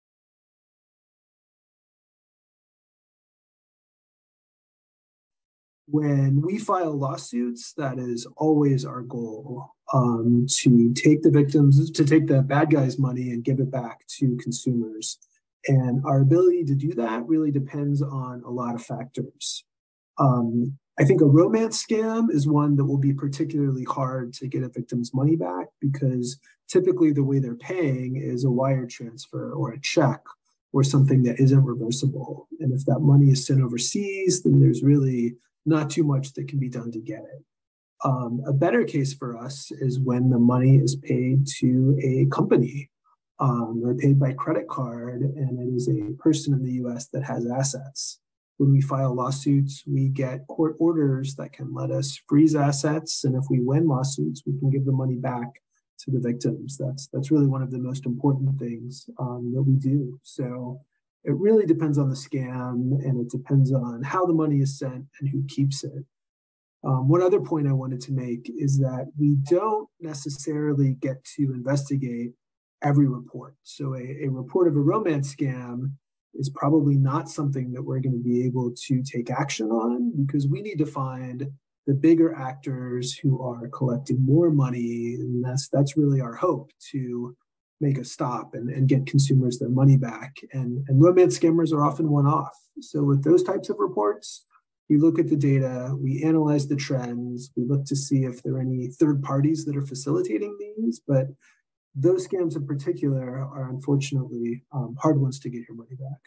How the FTC Recovers Money and Why it's Harder with Romance Scams
Benjamin Davidson, Attorney, Division of Marketing Practices, Federal
Trade Commission, explains how the FTC recovers money for victims of fraud and why it's harder to do so with romance scams.